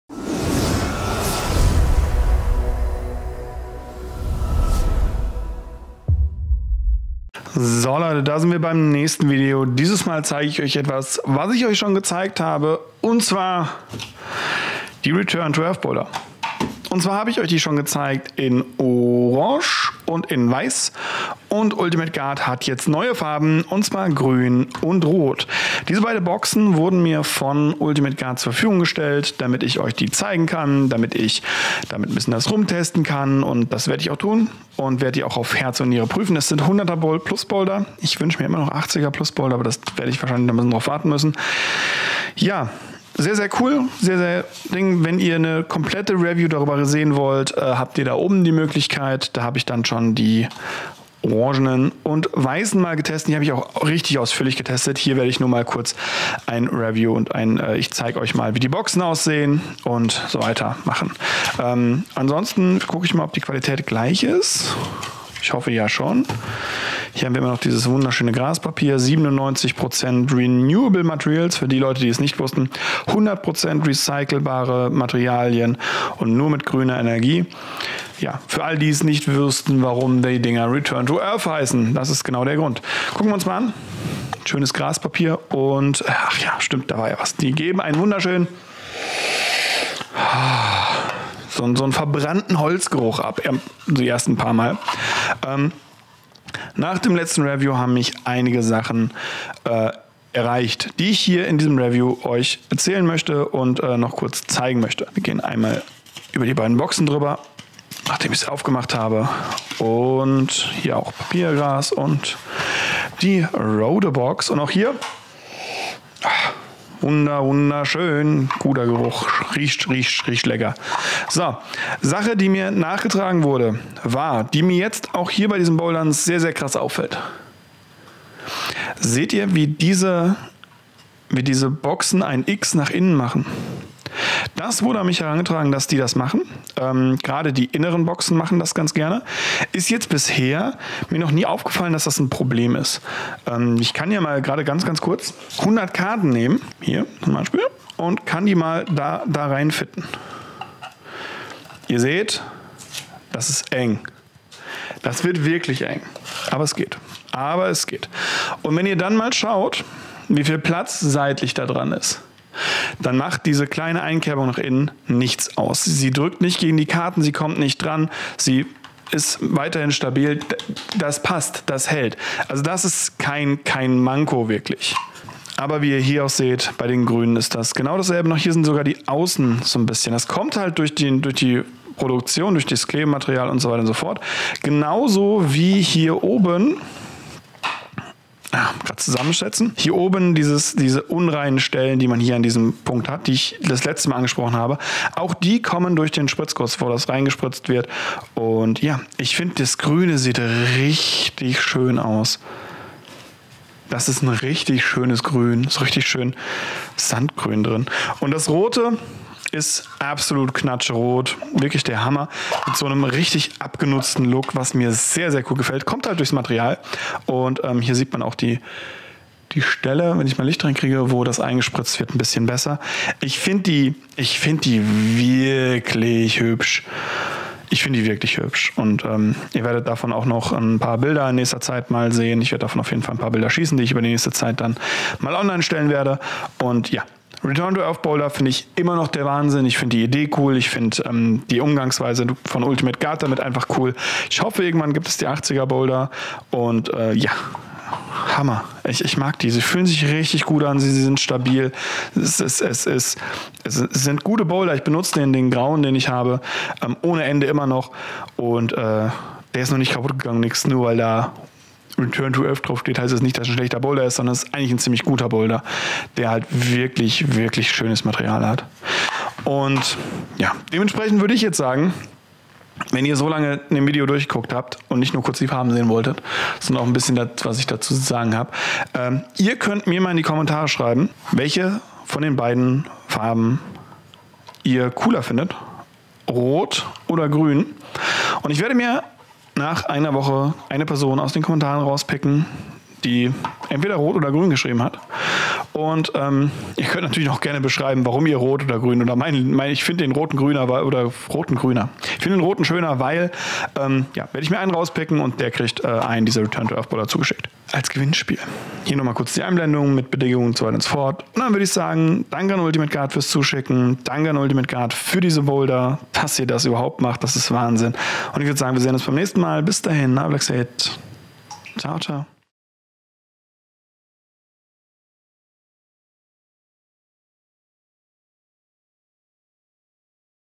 Blacky zeigt: die neuen Farben der Return to Earth Boulder von Ultimate Guard.
Heute zeige ich euch dank Ultimate Guard die neuen Return to Earth Boulder Farben, also Rot und Grün.

Sind die genauso gut von der Qualität und dem Umwelt Gedanken wie ihre orange und weißen Vorgänger?

Dazu kommt ein Gewinnspiel welches von Ultimate Guard unterstütz wird indem ihr einen RTE Boulder gewinnen könnt.
Was ihr tun müsst erfahrt ihr im Video!

0:00 Intro
1:02 Auspacken/Farben/Review
4:32 Suprice
5:12 Outro